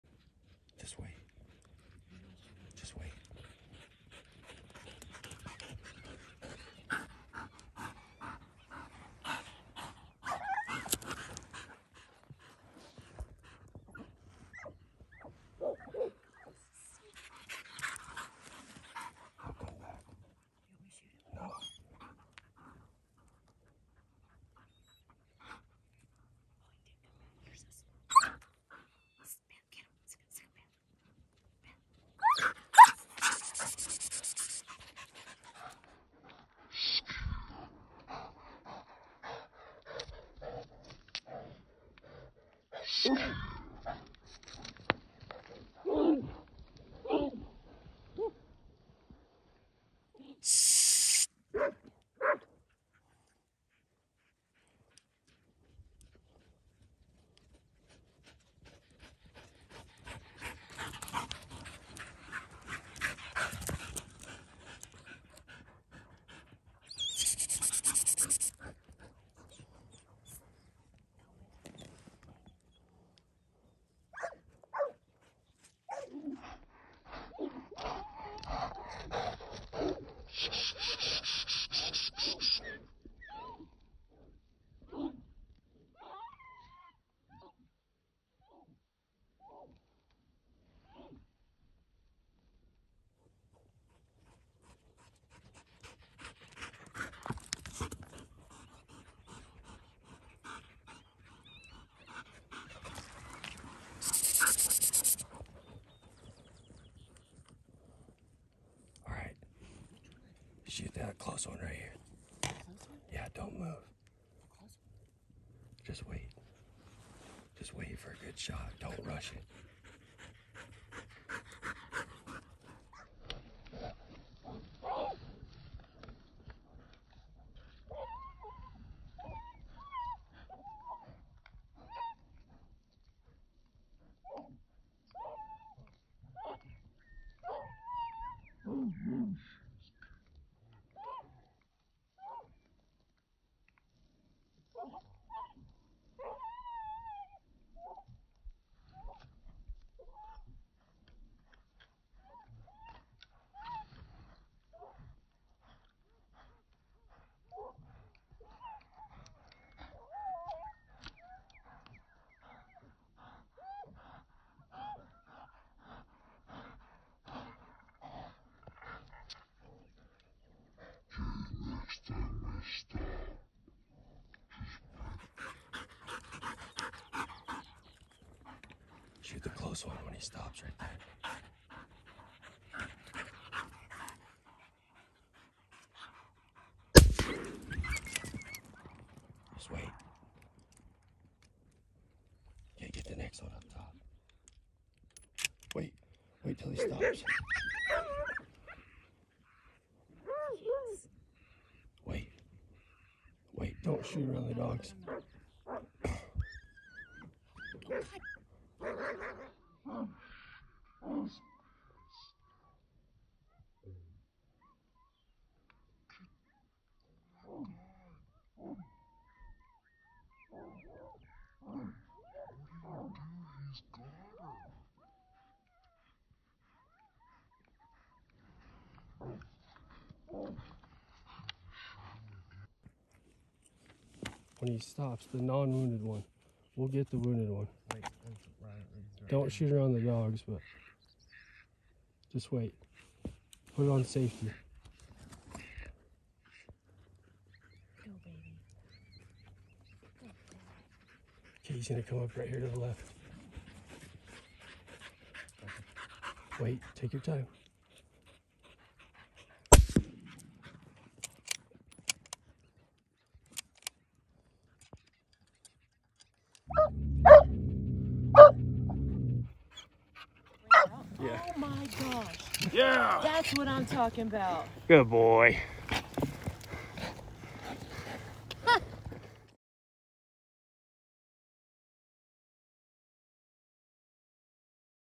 Decoy Doggin (epic triple)
Fun stand with some of my guided hunters on their first time hunting coyotes in Wyoming